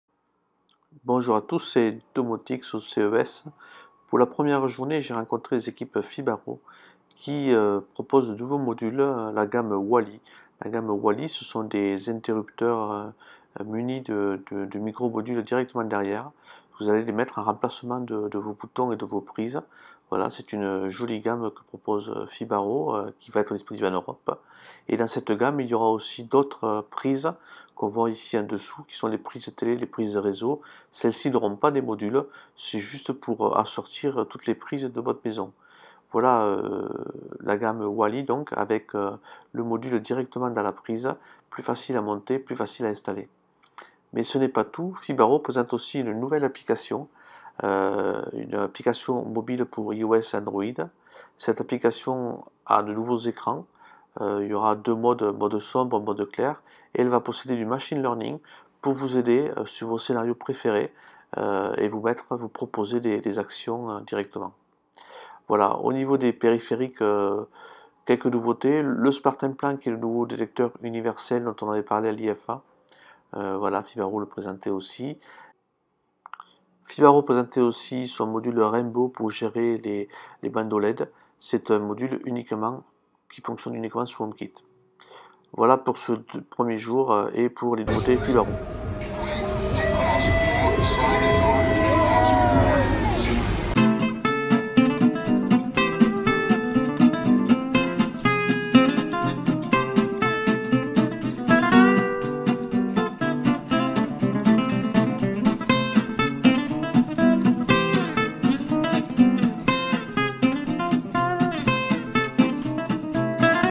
CES2019 - Les nouveautés FIBARO
Pour ce premier jour de CES, je vous propose de découvrir les nouveautés de la marque FIBARO. Au programme, une nouvelle application mobile et une nouvelle gamme de Périphériques un peu comme la gamme Celiane de LEGRAND ;-)

A bientot

Cdt
Domotics